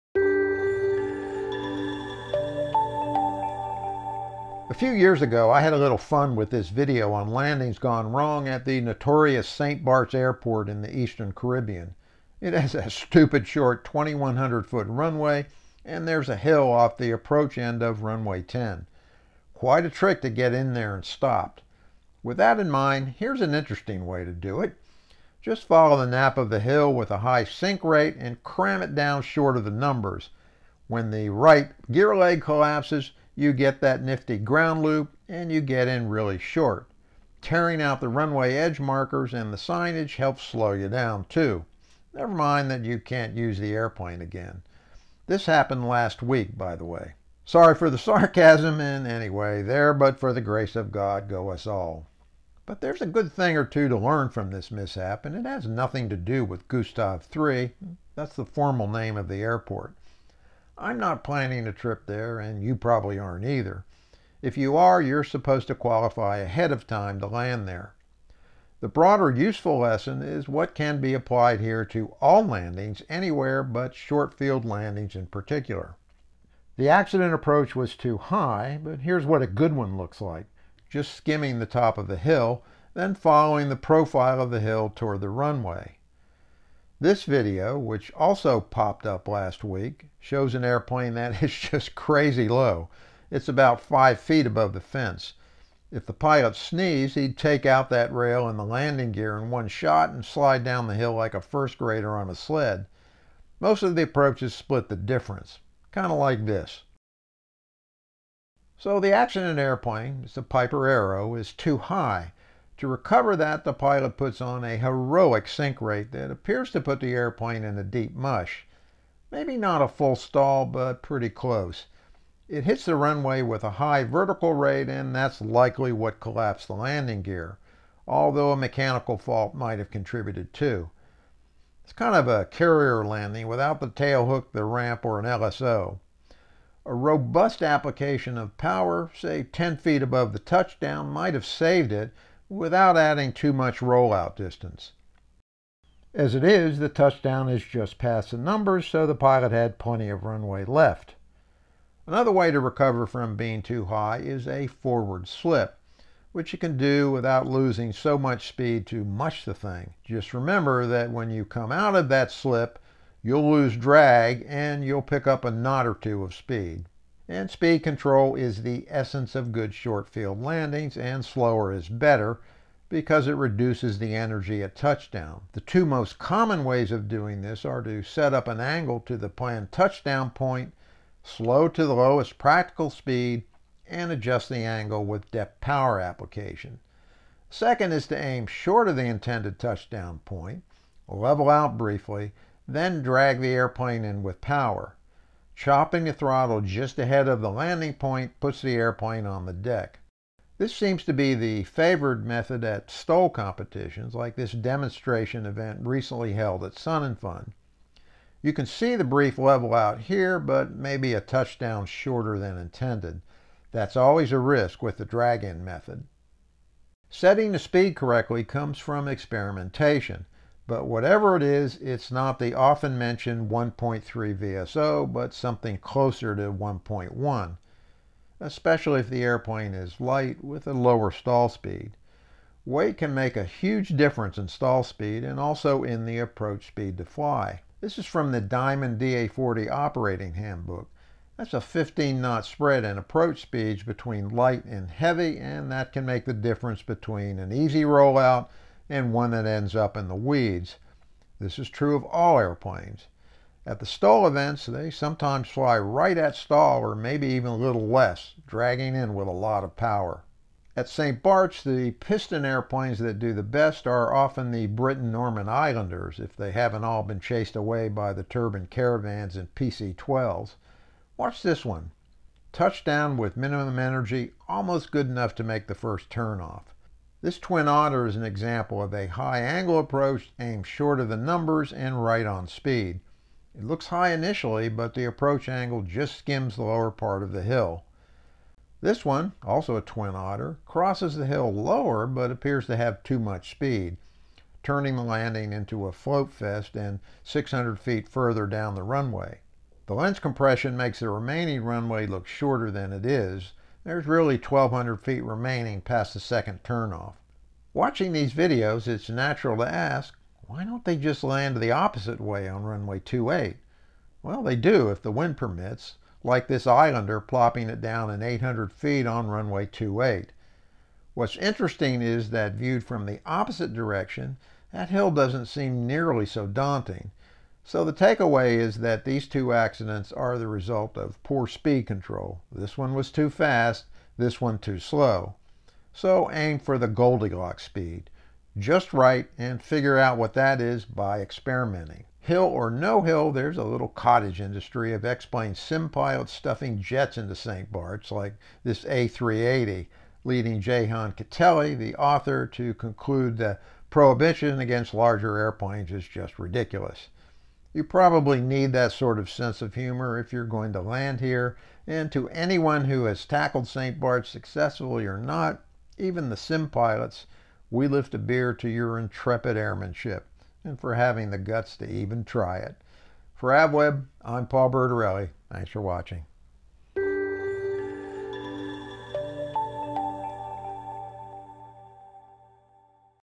Why This Landing Went Bad Part Deux
St. Barts Airport--actually Gustaf III-- on the Caribbean island of Saint Barthélemy is notorious for having a hill off the approach end of its 2100 foot runway. It's a challenge to land there and not everyone gets away unscathed. In this AVweb video, Paul Bertorelli looks at one landing that went wrong and why.